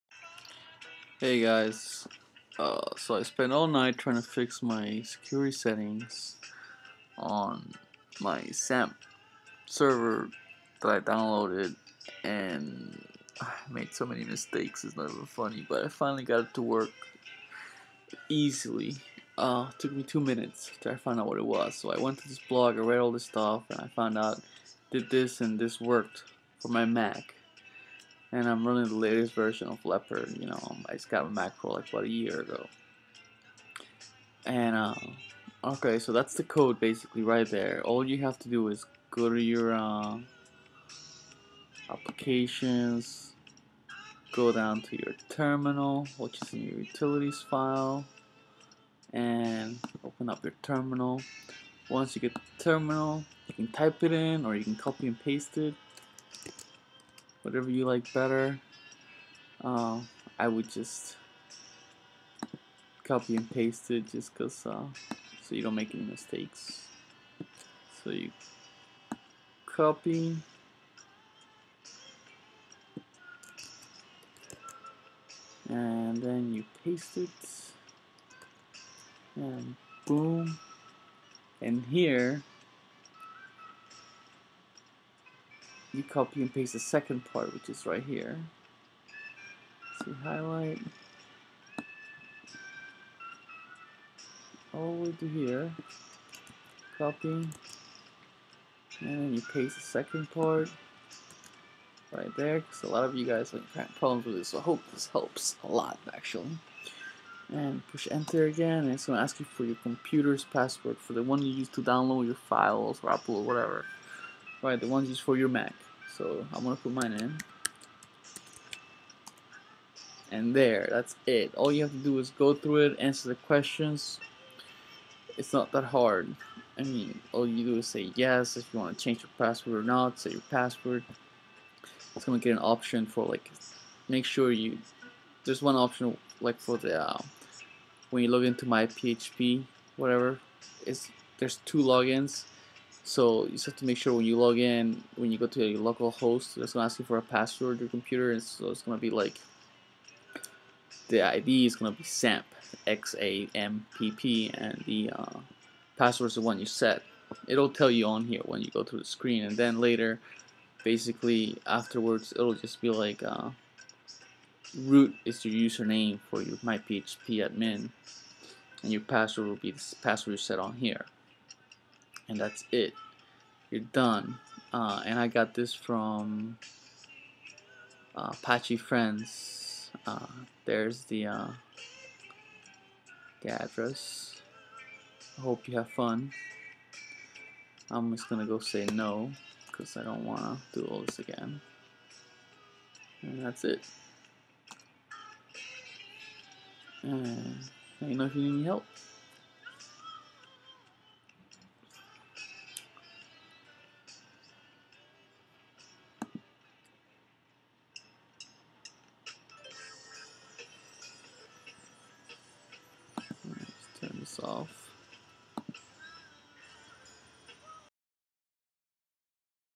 mysql error 1045 how to fix missing security xampp mysql password settings for Mac
so i downloaded xampp simple right then i had this security thing on phpmysqladmin was not working correctly it told me i had a security problem.
anyhow after much searching it was actually simple